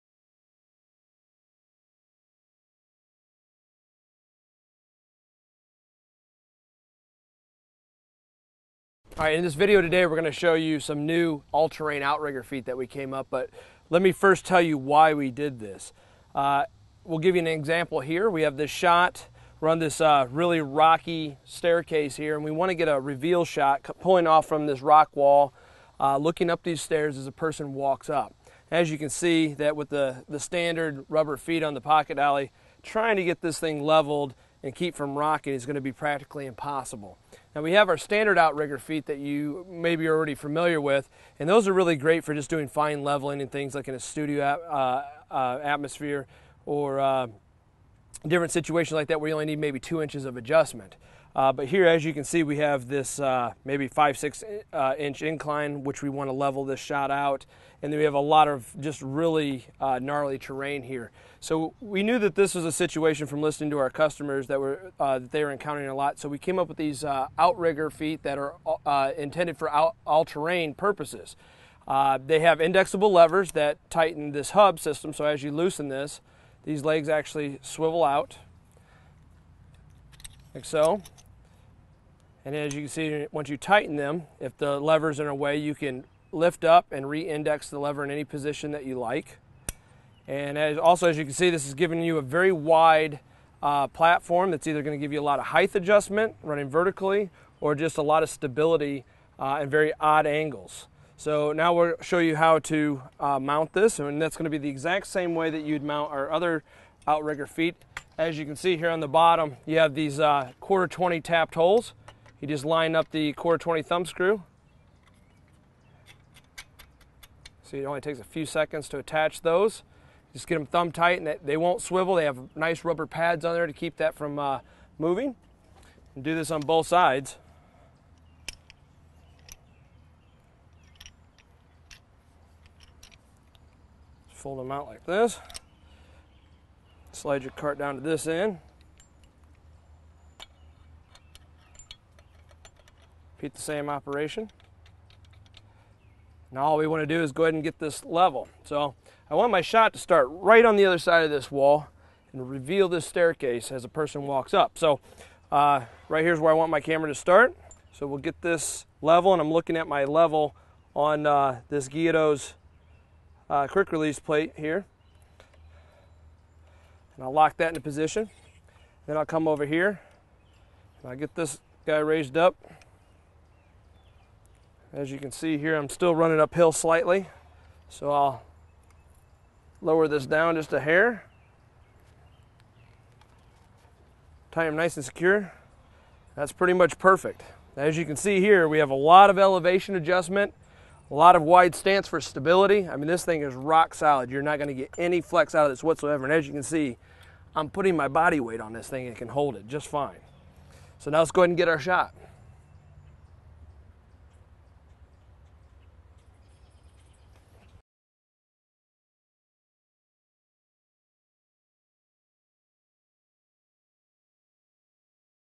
Kessler All-Terrain Outrigger Feet
feature a sturdy adjustable foot system for the Philip Bloom Pocket Dolly, CineSlider & Pocket Dolly sliders. These 360 degree adjustable legs are perfect for supporting our sliders in just about any situation. With indexable locking lever knobs you can easily tighten the legs into any position. Use these in place of the regular Outrigger Feet when more than 2" of adjustment is needed. or

A. F. Marcotec GmbH
Wiesenstraße 8
64347 Griesheim
Germany